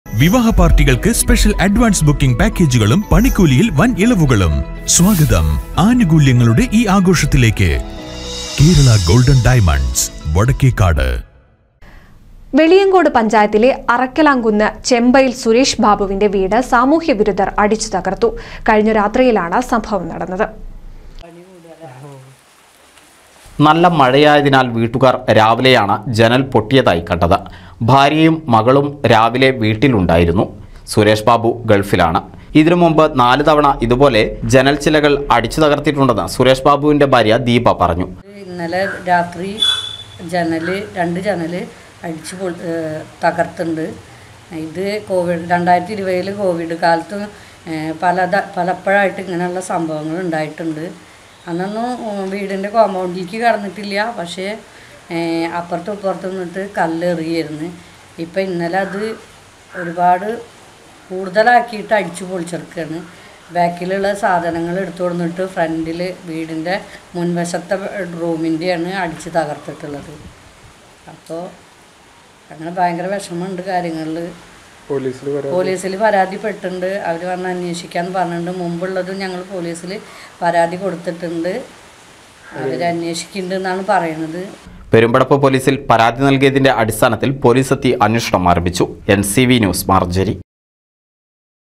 വീട് സാമൂഹ്യവിരുദ്ധര്‍ അടിച്ചുതകര്‍ത്തു
വെളിയങ്കോട് പഞ്ചായത്തിലെ  അറക്കലാകുന്ന്  ചെമ്പയില്‍ സുരേഷ് ബാബുവിന്റെ വീട് സാമൂഹ്യവിരുദ്ധര്‍ അടിച്ചുതകര്‍ത്തു. കഴിഞ്ഞ രാത്രിയിലാണ് സംഭവം നടന്നത്